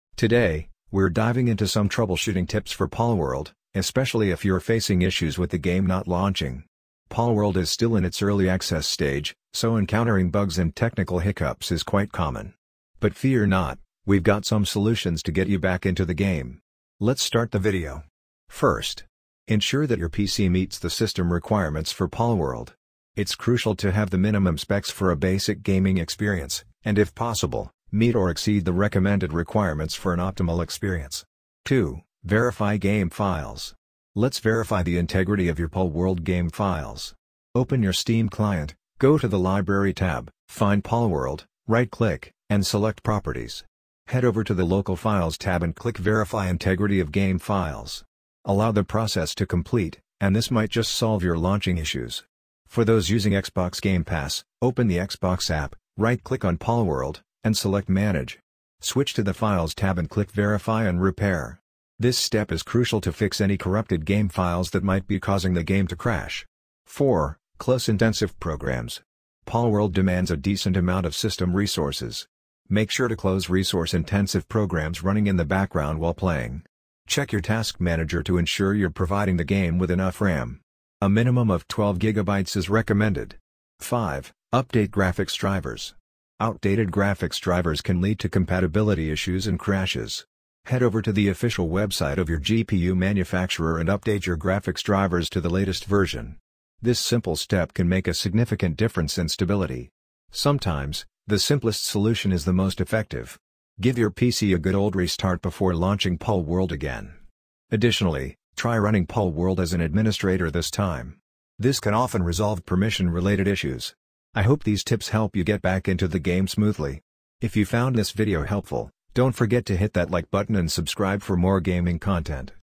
How to Fix Palworld keeps Crashing Error (Xbox,Pc) | Palworld Crashing and not launching Issues Fix
How to Fix Palworld keeps Crashing Error |  Palworld Crashing and not launching Issues Fixed

if you're facing issues with the game not launching. Palworld is still in its early access stage, so encountering bugs and technical hiccups is quite common. But fear not, we've got some solutions to get you back into the game.